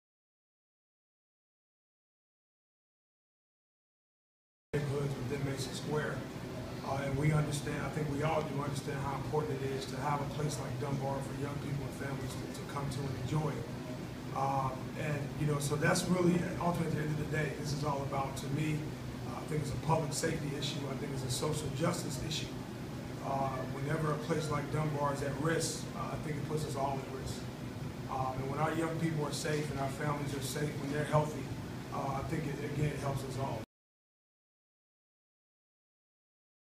Kirk Smith talks about the Dunbar Community Center
Kirk Smith,President and CEO of the YMCA of Greater Springfield and others are hopeful that the Dunbar Community Center can be saved.